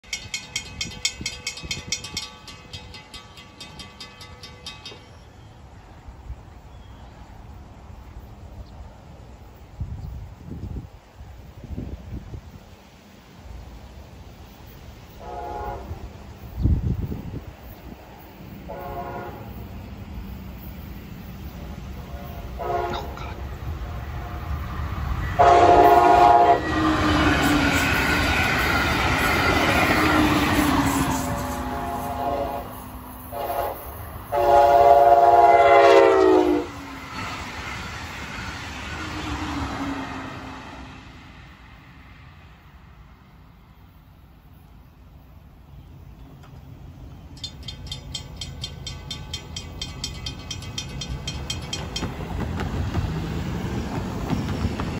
BLF TRAINS 722 blue &701 Orlando wrap meet @￼ 9:30am 10-2-23
BLF 722 southbound blf engines 117&113

BLF 701 northbound blf engines 120&121